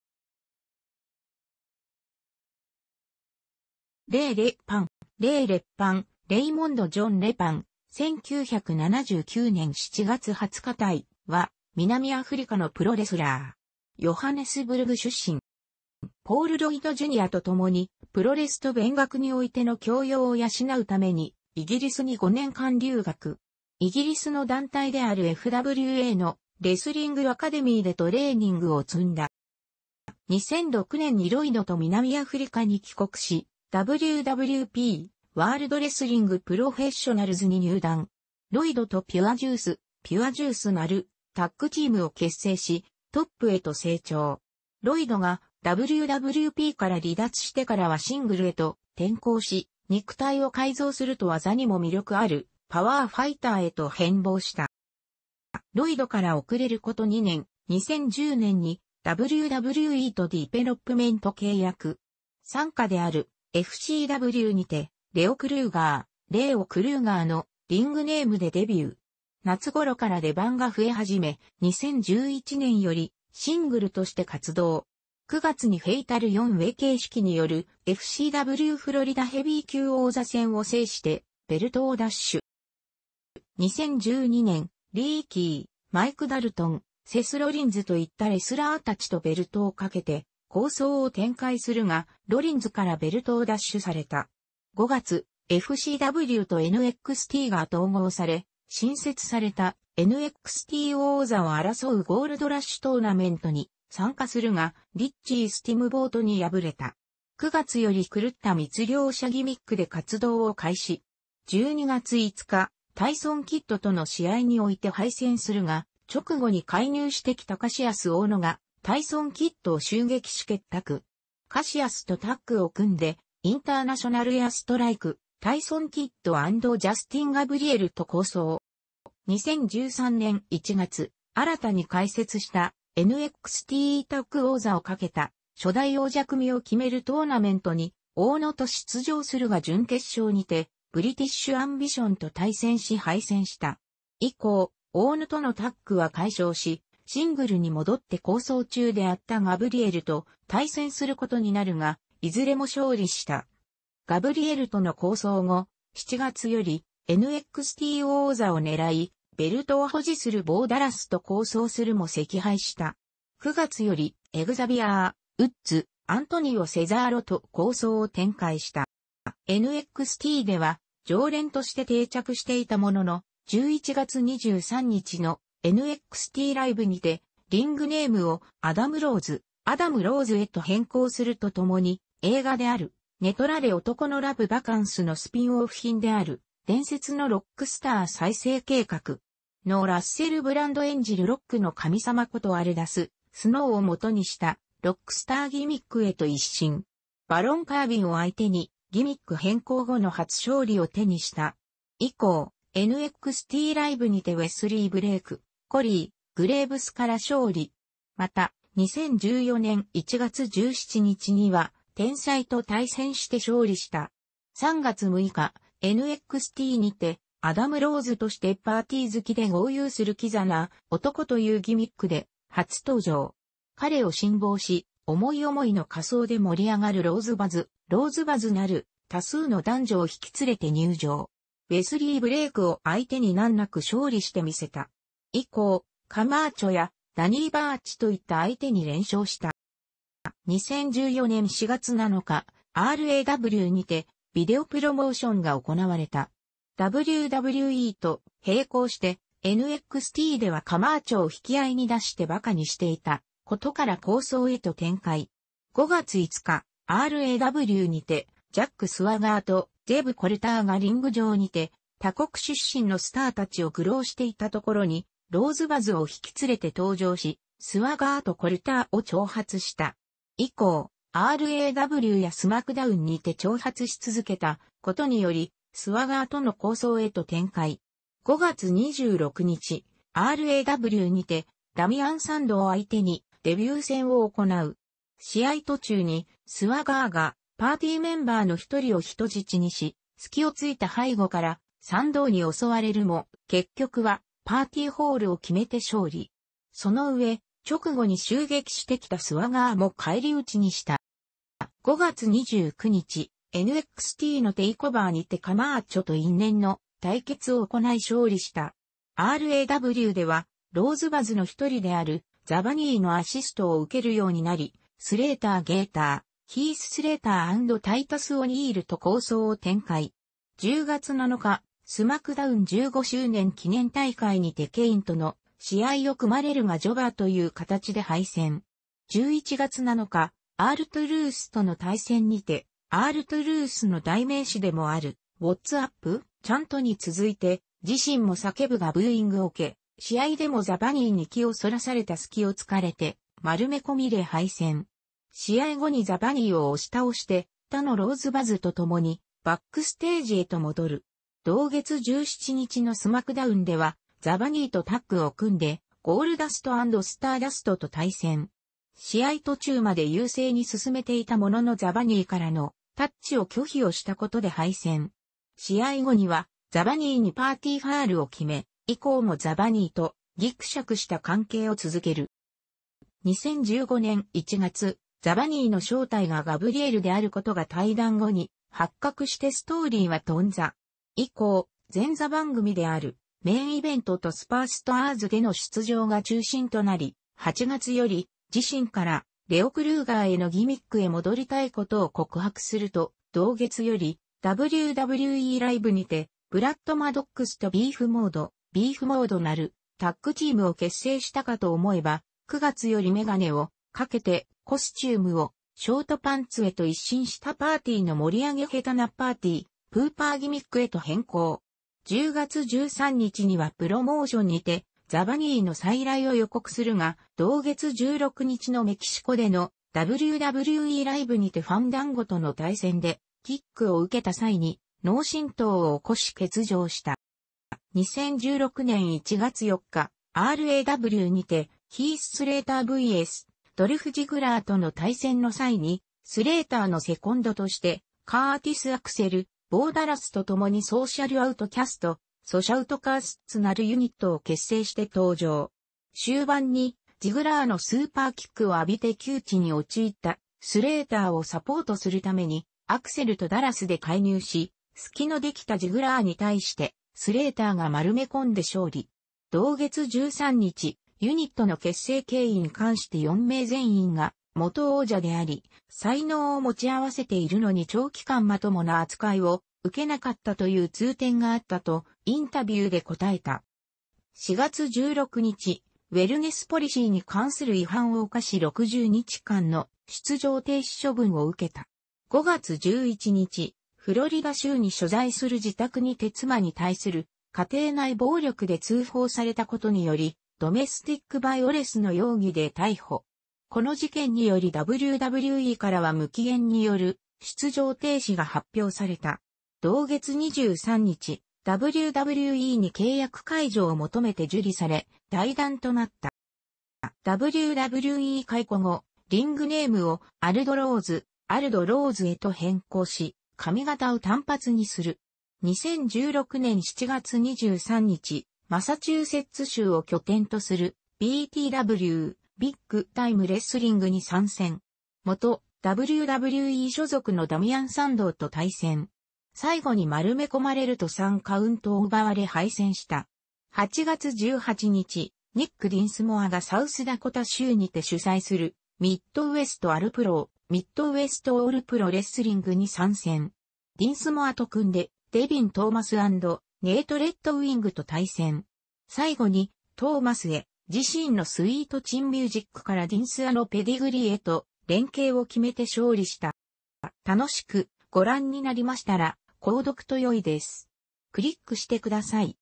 レイ・レッパン
レイ・レッパン（"Raymond John Leppan"、1979年7月20日-）は、南アフリカのプロレスラー。ヨハネスブルグ出身。

ポール・ロイド・ジュニアと共にプロレスと勉学においての教養を養うためにイギリスに5年間留学。イギリスの団体であるFWAのレスリング・アカデミーでトレーニングを積んだ。

2006年にロイドと南アフリカに帰国し、WWP（World Wrestling Professionals）に入団。ロイドとピュア・ジュース（Pure Juice）なるタッグチームを結成し、トップへと成長。ロイドがWWPから離脱してからはシングルへと転向し、肉体を改造すると技にも魅力あるパワーファイターへと変貌した。

ロイドから遅れること2年、2010年にWWEとディペロップメント契約。傘下であるFCWにてレオ・クルーガー（"Leo Kruger"）のリングネームでデビュー。夏頃から出番が増え始め、2011年よりシングルとして活動。9月にフェイタル4ウェイ形式によるFCWフロリダヘビー級王座戦を制してベルトを奪取。

2012年、リーキー、マイク・ダルトン、セス・ロリンズといったレスラーたちとベルトを賭けて抗争を展開するが、ロリンズからベルトを奪取された。5月、FCWとNXTが統合され新設されたNXT王座を争うゴールドラッシュ・トーナメントに参加するがリッチー・スティムボートに敗れた。9月より狂った密猟者ギミックで活動を開始。12月5日、タイソン・キッドとの試合において敗戦するが、直後に介入してきたカシアス・オーノがタイソン・キッドを襲撃し結託。カシアスとタッグを組んでインターナショナル・エアストライク（タイソン・キッド & ジャスティン・ガブリエル）と抗争。

2013年1月、新たに開設したNXTタッグ王座を懸けた初代王者組を決めるトーナメントにオーノと出場するが準決勝にてブリティッシュ・アンビションと対戦し敗戦した。以降、オーノとのタッグは解消し、シングルに戻って抗争中であったガブリエルと対戦することになるが、いずれも勝利した。ガブリエルとの抗争後、7月よりNXT王座を狙い、ベルトを保持するボー・ダラスと抗争するも惜敗した。9月よりエグザビアー・ウッズ、アントニオ・セザーロと抗争を展開した。

NXTでは常連として定着していたものの、11月23日のNXT Liveにてリングネームをアダム・ローズ（"Adam Rose"）へと変更すると共に、映画である寝取られ男のラブ♂バカンスのスピンオフ作品である伝説のロックスター再生計画！のラッセル・ブランド演じるロックの神様ことアルダス・スノーを基にしたロックスターギミックへと一新。バロン・カービンを相手にギミック変更後の初勝利を手にした。以降、NXT Liveにてウェスリー・ブレイク、コリー・グレイブスから勝利。また、2014年1月17日にはテンサイと対戦して勝利した。3月6日、NXTにてアダム・ローズとしてパーティー好きで豪遊するキザな男というギミックで初登場。彼を信望し、思い思いの仮装で盛り上がるローズバズ（Rosebuds）なる多数の男女を引き連れて入場。ウェスリー・ブレイクを相手に難なく勝利してみせた。以降、カマーチョやダニー・バーチといった相手に連勝した。

2014年4月7日、RAWにてビデオプロモーションが行われた。WWEと並行してNXTではカマーチョを引き合いに出して馬鹿にしていた事から抗争へと展開。5月5日、RAWにてジャック・スワガーとゼブ・コルターがリング上にて他国出身のスター達を愚弄していたところにローズ・バズを引き連れて登場し、スワガーとコルターを挑発した。以降、RAWやSmackDownにて挑発し続けた事によりスワガーとの抗争へと展開。5月26日、RAWにてダミアン・サンドウを相手にデビュー戦を行う。試合途中にスワガーがパーティーメンバーの一人を人質にし、隙を突いた背後からサンドウに襲われるも、結局はパーティー・フォールを決めて勝利。そのうえ、直後に襲撃してきたスワガーも返り討ちにした。

5月29日、NXTのTakeoverにてカマーチョと因縁の対決を行い勝利した。RAWではローズバズの一人であるザ・バニーのアシストを受けるようになり、スレイター・ゲイター（ヒース・スレイター & タイタス・オニール）と抗争を展開。10月7日、SmackDown15周年記念大会にてケインとの試合を組まれるがジョバーという形で敗戦。11月7日、Rトゥルースとの対戦にてRトゥルースの代名詞でも...